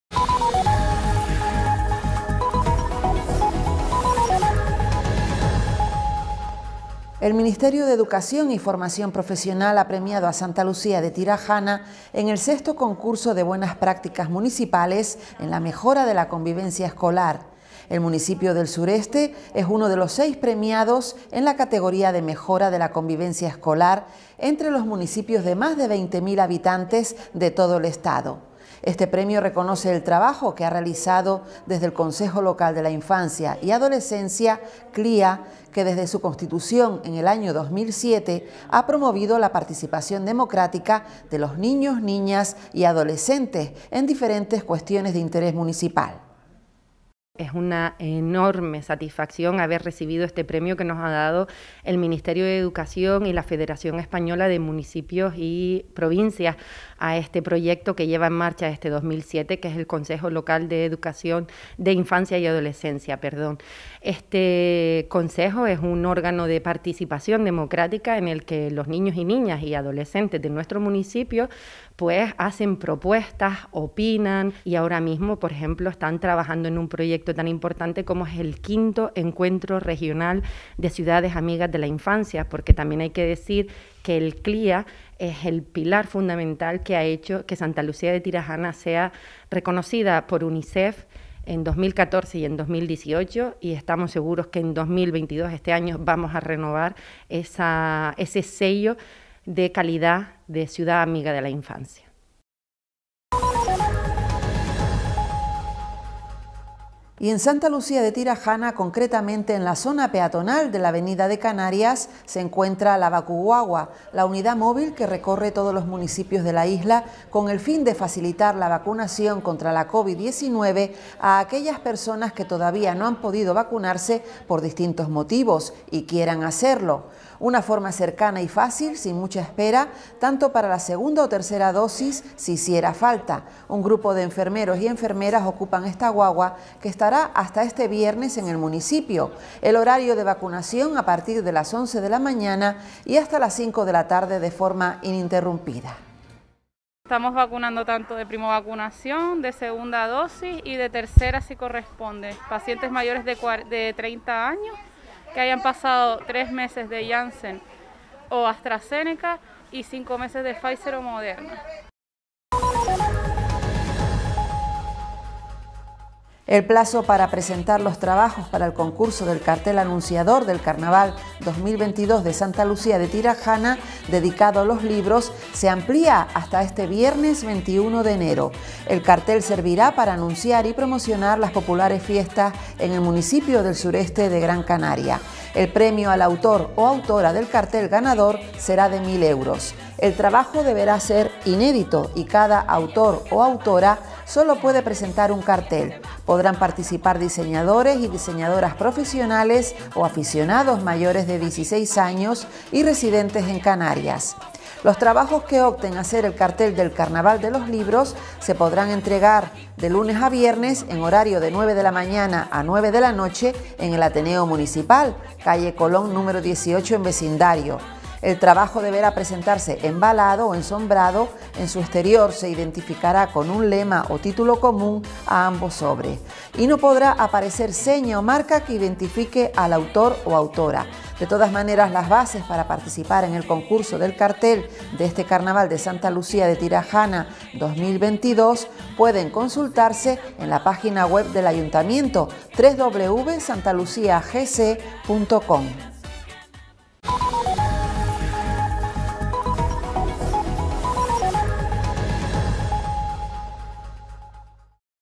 INFORMATIVO SANTA LUCÍA WEB - 18 01 2022
Informativo del municipio de Santa Lucía:
1-El Ministerio de Educación premia al CLIA de Santa Lucía.
2-La Vacuguagua en la Avenida de Canarias de Vecindario.
3-Ampliación del plazo para participar en la elaboración del Cartel de Carnaval de Santa Lucía.